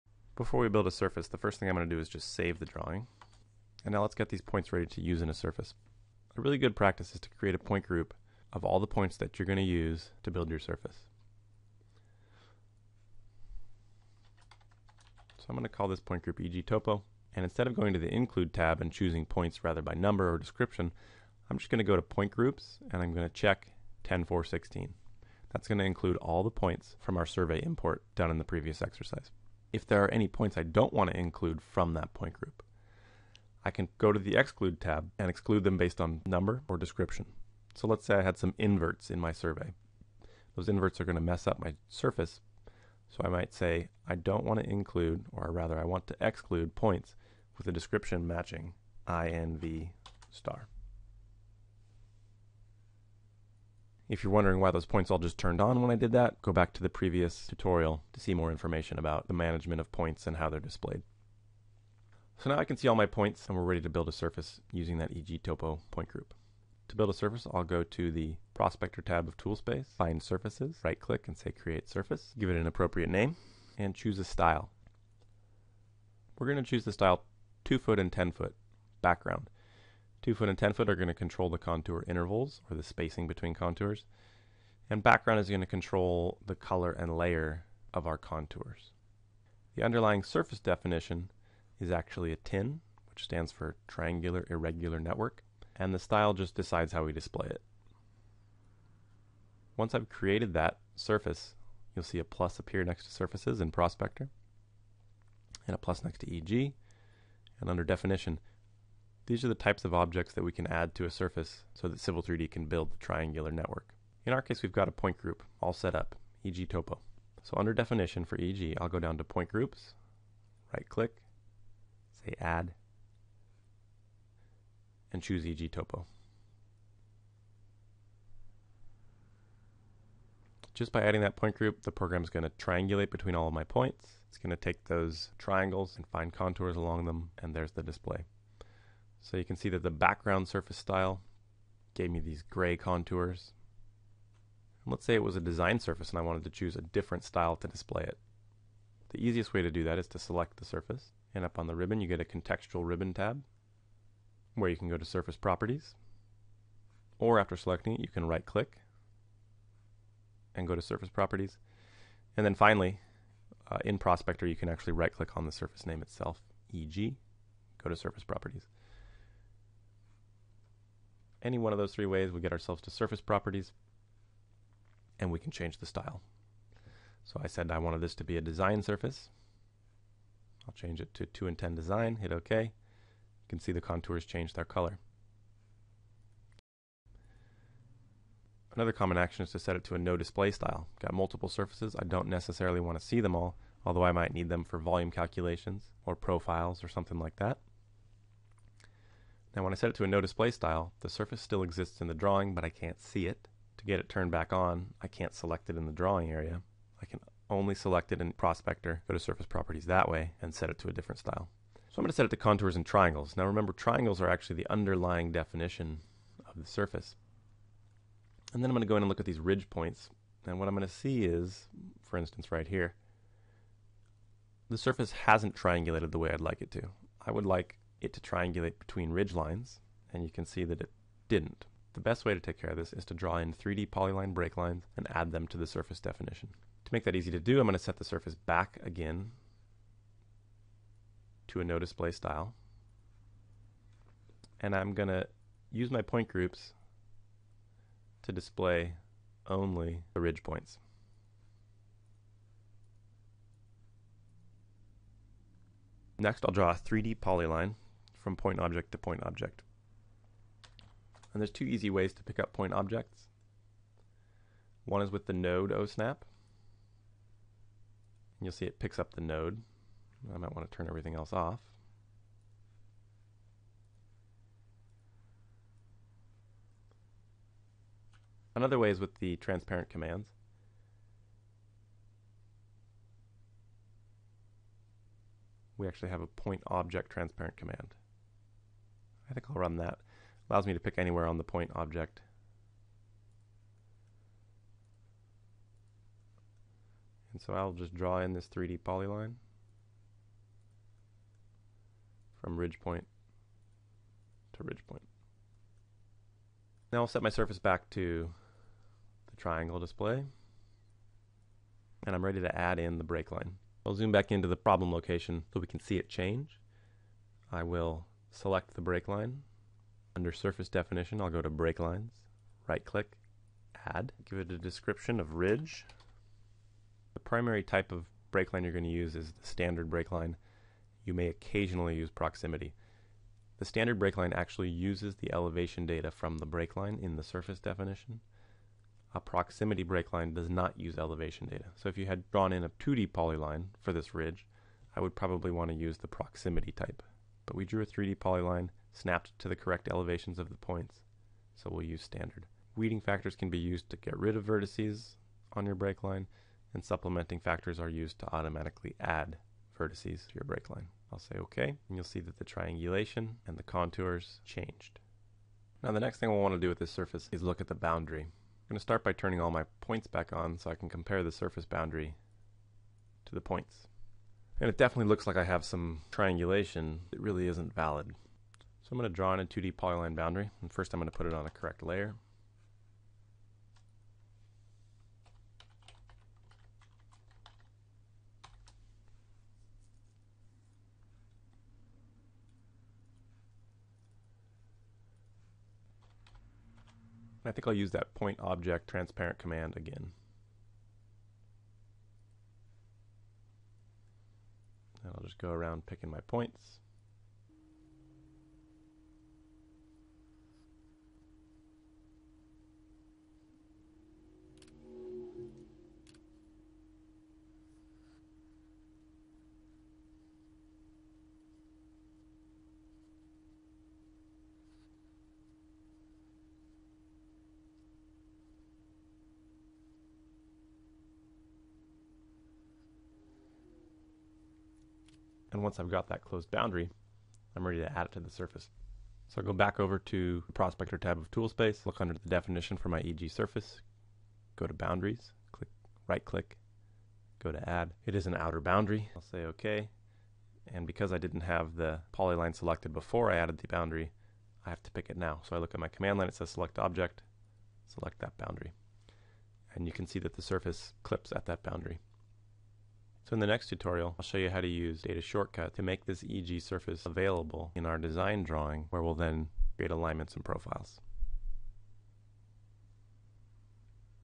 Civil 3D Tutorial 2: Surfaces
Civil 3D Introduction Tutorial: Part 2 in this series introduces how to create surfaces in AutoCAD Civil 3D.